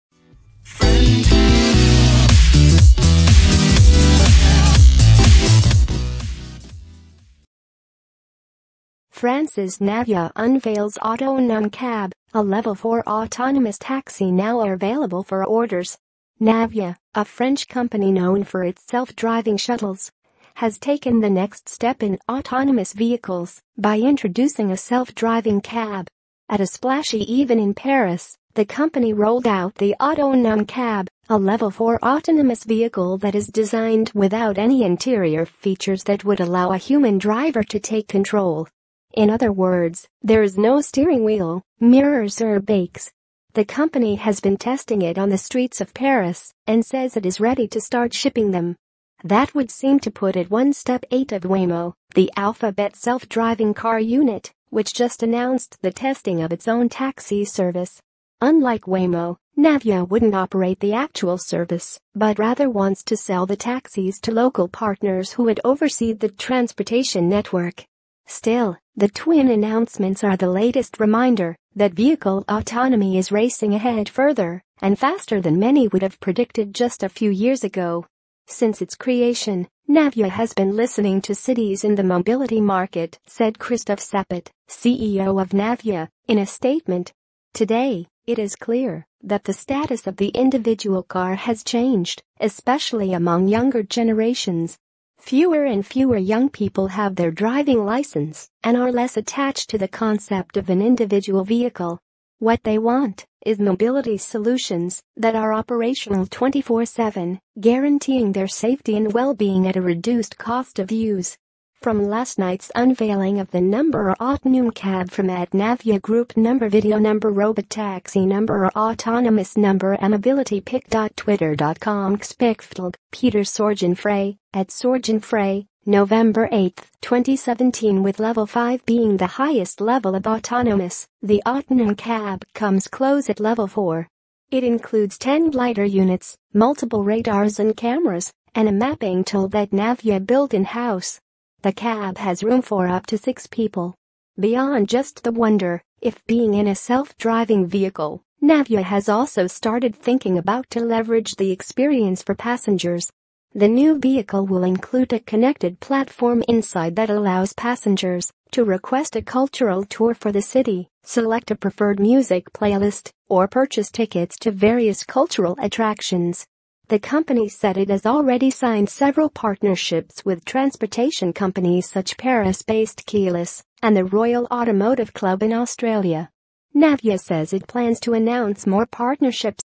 France’s Navya unveils Autonum Cab, a Level 4 autonomous taxi now available for orders
Navya, a French company known for its self-driving shuttles, has taken the next step in autonomous vehicles by introducing a self-driving cab.

At a splashy even in Paris, the company rolled out the Autonum Cab, a Level 4 autnomous vehicle that is designed without any interior features that would allow a human driver to take control. In other words, there is no steering wheel, mirrors or bakes.

The company has been testing it on the streets of Paris, and says it is ready to start shipping them. That would seem to put it one step ahed of Waymo, the Alphabet self-driving car unit, which just announced the testing of its own taxi service. Unlike Waymo, Navya wouldn’t operate the actual service, but rather wants to sell the taxis to local partners who would oversee the transportation network.